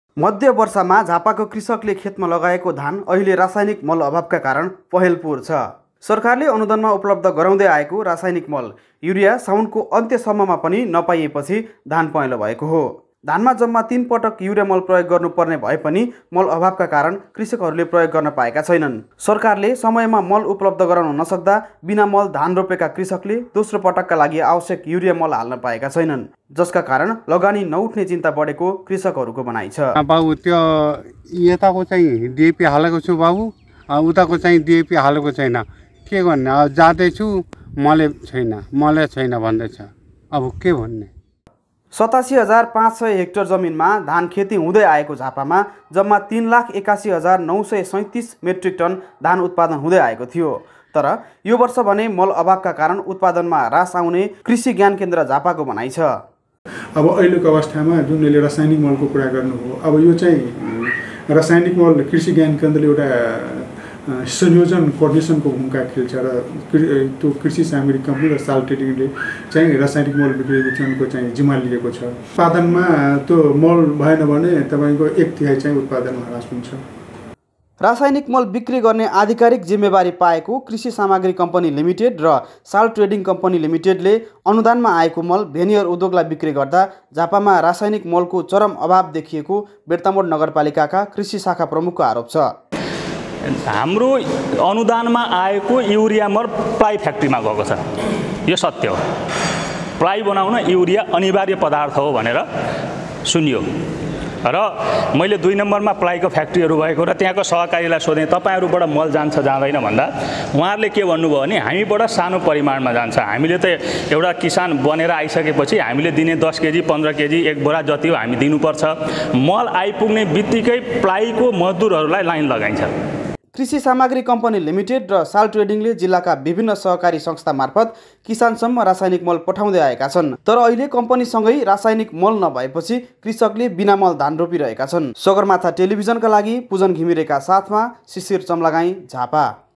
मल अभावले धान उत्पादन घट्ने | Jhapa | Sagarmatha Report
अन्नका भण्डारका रुपमा चिनीने झापामा अन्य वर्षको तुलनामा यो वर्ष धान उत्पादनमा एक तिहाई परिमाणले कमी आउने प्रक्षेपण गरिएको छ । धानमा लगाउने रासायनिक मलको चरम अभावका कारण धान उत्पादनमा घट्ने भएको हो ।